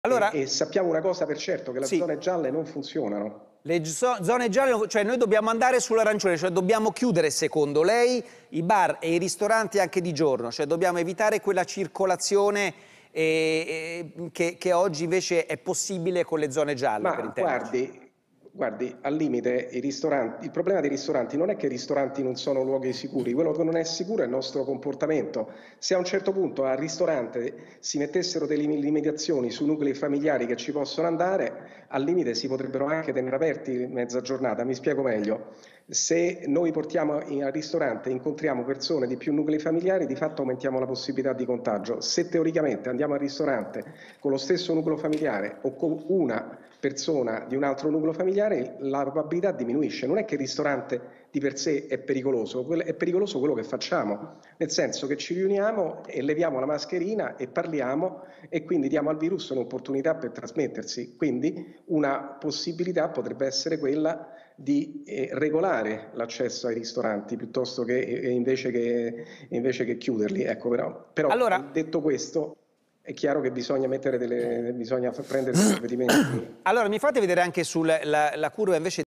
Covid, Crisanti: “Bisognerebbe regolare l’accesso ai ristoranti, anziché chiuderli"
Andrea Crisanti: “Sappiamo una cosa per certo: le zone gialle non funzionano. Non è che i ristoranti non sono luoghi sicuri ma non è sicuro il nostro comportamento. Se ci fossero limitazioni ai nuclei familiari con cui andiamo al ristorante, ci sarebbero meno possibilità di contagio. Bisognerebbe regolare l’accesso ai ristoranti, anziché chiuderli”.